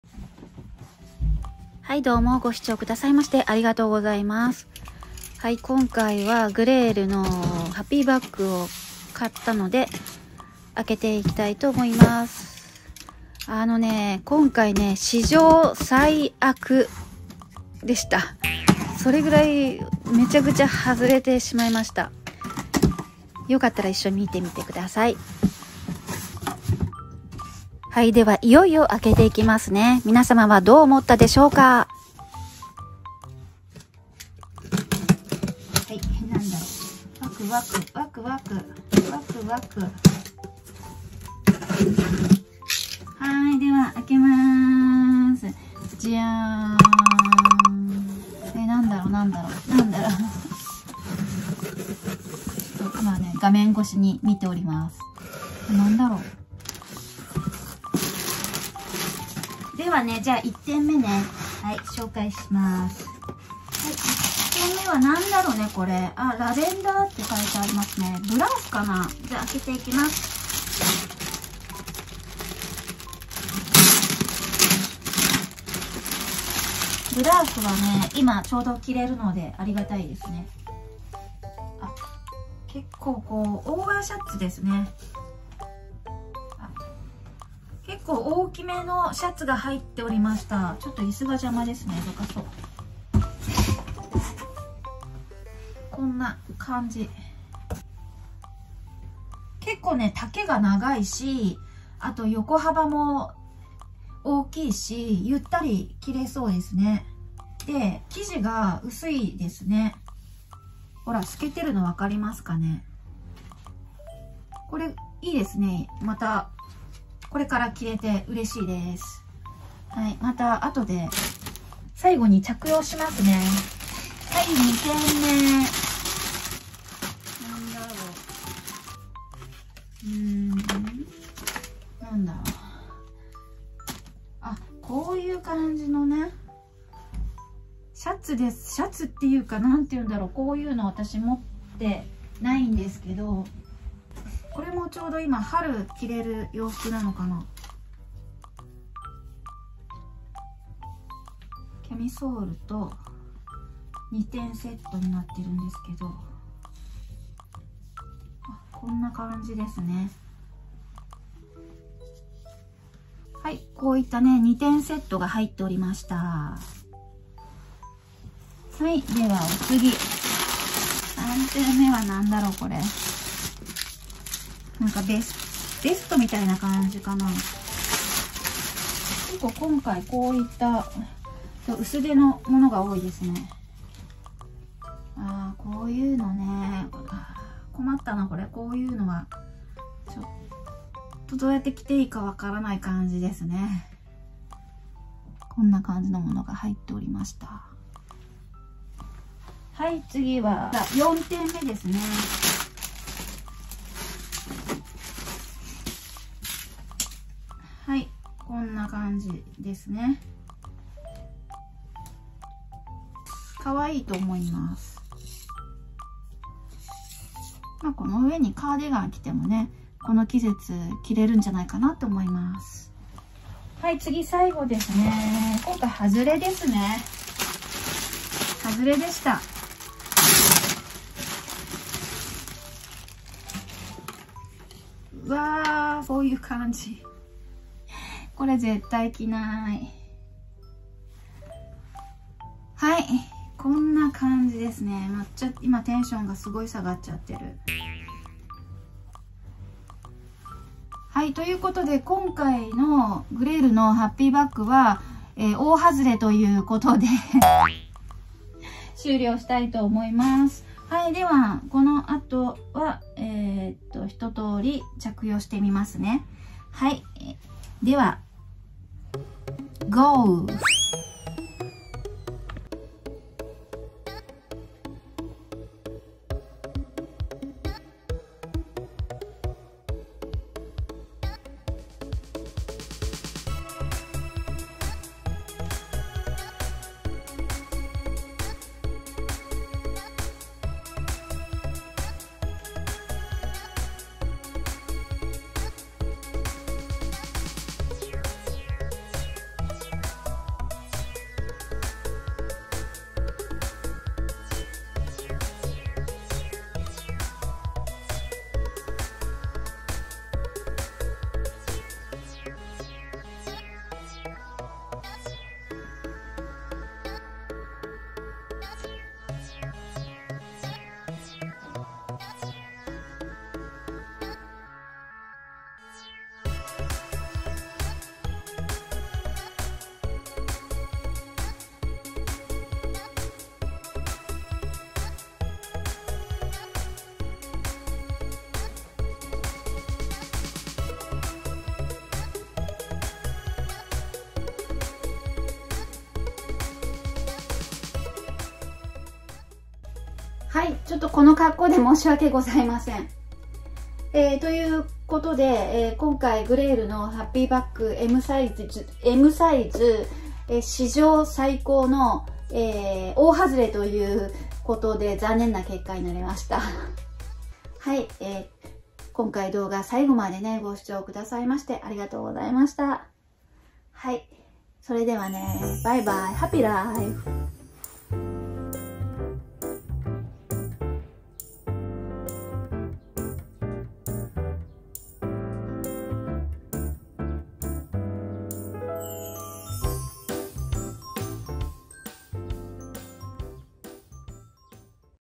【GRL】40代グレイル５点入り１９９９円福袋買ったら😢凄い結果に‼️
【少しでも参考になりますように】
骨格ウェーブ
身長152cm
体重47kg（変動あり）
足のサイズ20～21cm

今日の洋服もGRLです( ˆ♡ˆ )

【トップス】
フリルハイネックブラウス[tu161]
【ジャンパースカート】
レザージャンパースカート[tu85]

【GRL】
ちょっとGRL興味あるかも～‼️
と思った方がいましたら🔻🔻🔻

※ 週末やキャンペーン中なら送料無料でおすすめですよ～(^-^)/